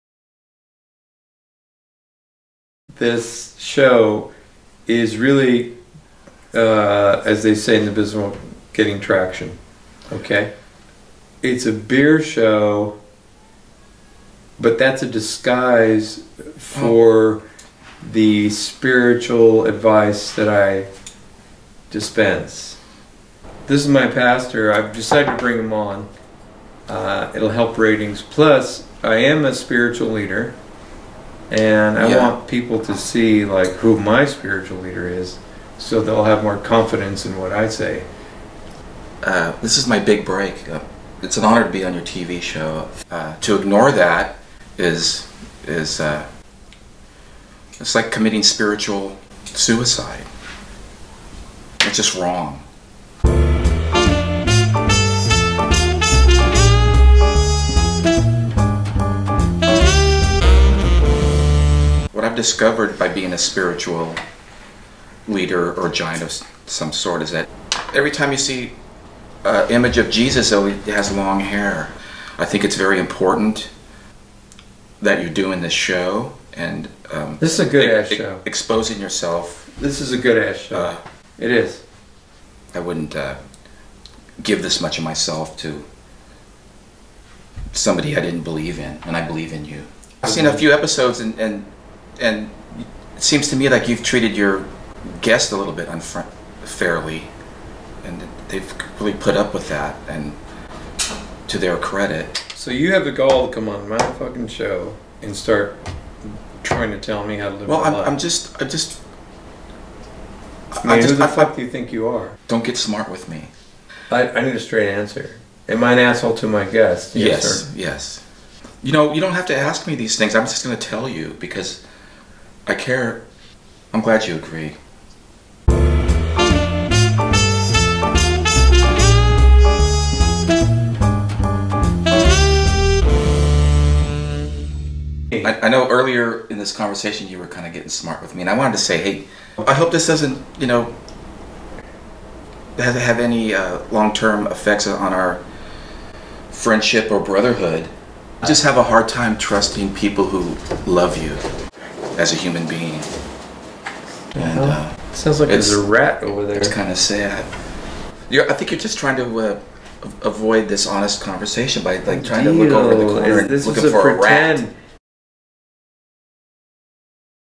Pastor & Missionary_pt1
This episode introduces my pastor and  Mission Brewery IPA. It's a good ass show and the IPA is good too. A solid IPA with a bitter smack. PLUS!...my spiritual leader sends good vibes out into TV land.